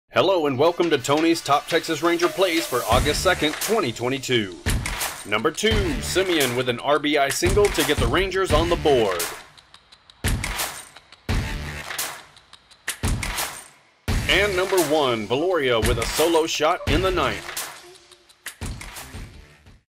⚾⚾⚾ MLB - Texas Rangers Top Plays - August 2, 2022 ⚾⚾⚾
My choices for top plays for the Texas Rangers game on August 2, 2022, against the Baltimore Orioles at Globe Life Field.

#2 Semien with an RBI single to get the Rangers on the board.
#1 Viloria with a solo shot in the 9th.